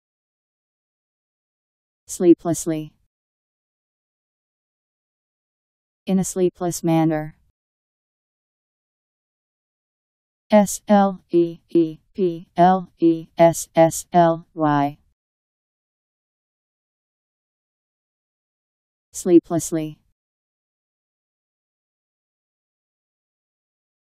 Sleeplessly Meaning
Video shows what sleeplessly means. In a sleepless manner..  Sleeplessly Meaning. How to pronounce, definition audio dictionary. How to say sleeplessly. Powered by MaryTTS, Wiktionary